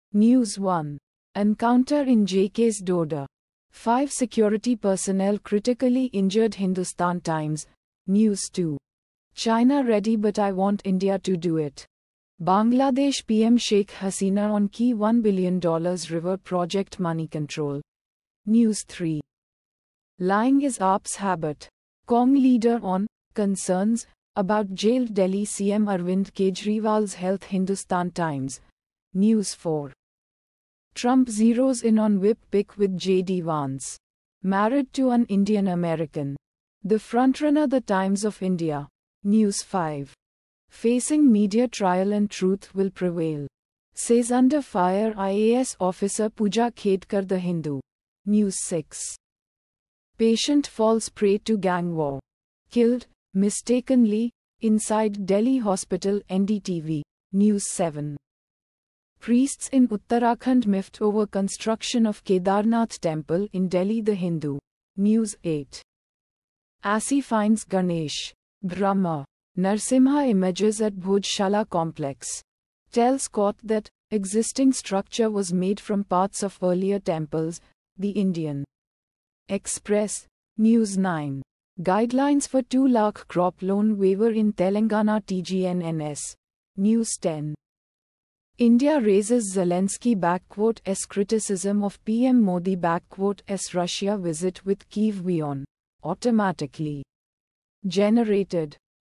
News Headlines (India)- 17/07/2024,02:46 AM GMT+5:30
News 1:Encounter in J-K's Doda: 5 security personnel critically injured - Hindustan Times
News 2:'China ready but I want India to do it': Bangladesh PM Sheikh Hasina on key $1 billion river project - Moneycontrol
News 3:‘Lying is AAP’s habit': Cong leader on ‘concerns’ about jailed Delhi CM Arvind Kejriwal's health - Hindustan Times
News 4:Trump zeroes in on Veep pick with JD Vance, married to an Indian-American, the frontrunner - The Times of India
News 5:Facing media trial and truth will prevail, says under-fire IAS officer Puja Khedkar - The Hindu
News 6:Patient Falls Prey To Gang War, Killed 'Mistakenly' Inside Delhi Hospital - NDTV
News 7:Priests in Uttarakhand miffed over construction of Kedarnath temple in Delhi - The Hindu
News 8:ASI finds Ganesh, Brahma, Narasimha images at Bhojshala complex, tells court that ‘existing structure was made from parts of earlier temples’ - The Indian Express
News 9:Guidelines for 2 lakh Crop Loan Waiver in Telangana - Tgnns
News 10:India raises Zelensky`s criticism of PM Modi`s Russia visit with Kyiv - WION

Automatically Generated...